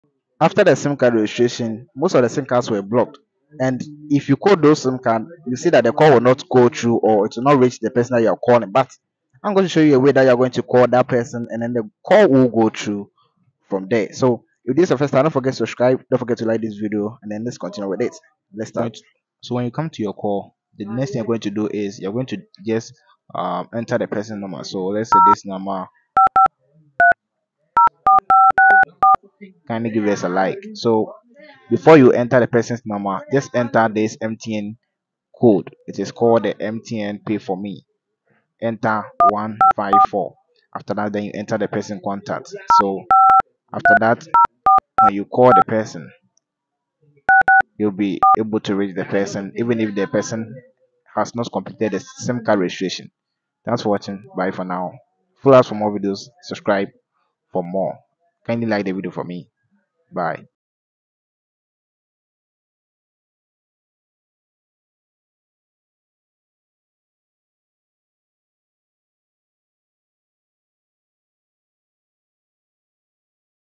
How to call blocked SIM cards after SIM REGISTRATION - Works 💯
This is MTN code that you will add to the contacts before calling

‼️Will really Help in case of emergency 🚨